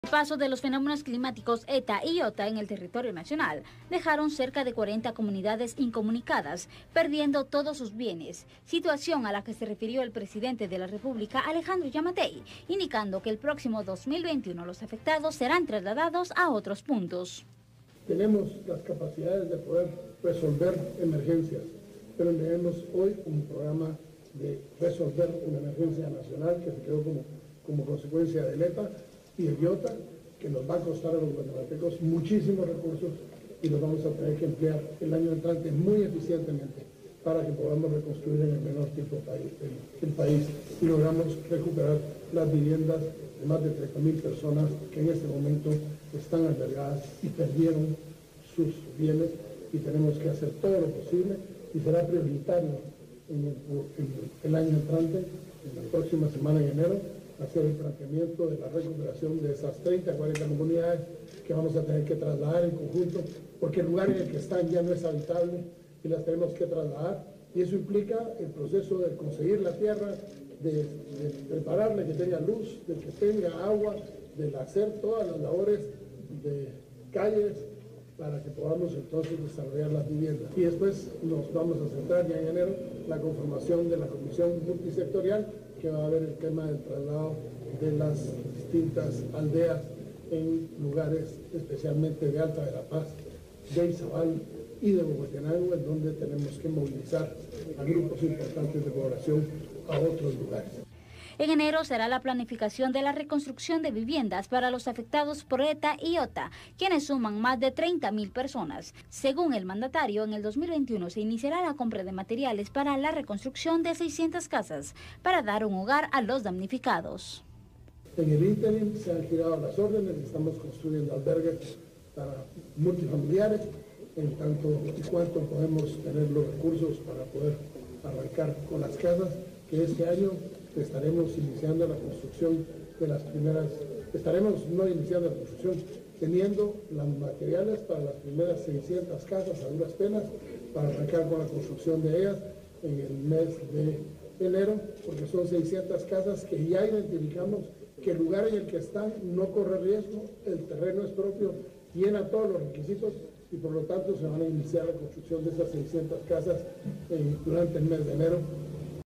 Reconstrucción de 600 viviendas a damnificados por ''Eta'' e ''Iota''
Presidente de la República, Alejandro Giammattei, brindó detalles

El presidente de la República, Alejandro Giammattei, brindó detalles sobre la reconstrucción de las primeras 600 casas para las personas damnificadas por las tormentas tropicales "Eta" e "Iota".